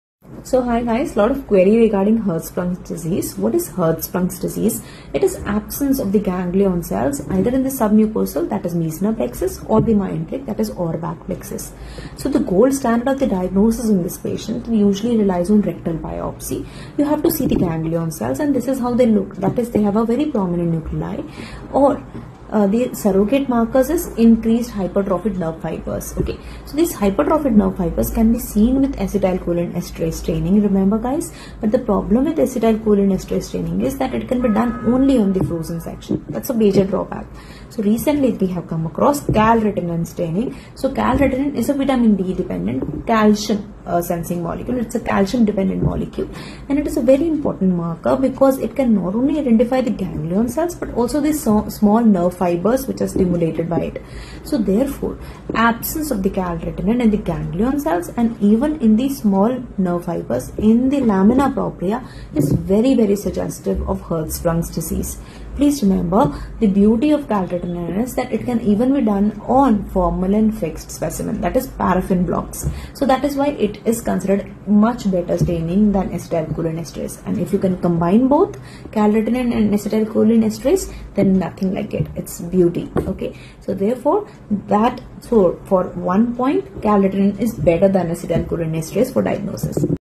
Calretinin remains a reliable marker for Hirschsprung diagnosis
As of the latest guidelines and literature (up to 2024), Calretinin remains a reliable marker for Hirschsprung diagnosis:

Negative staining = supports diagnosis of Hirschsprung disease.

Positive staining = suggests presence of ganglion cells (i.e., normal innervation).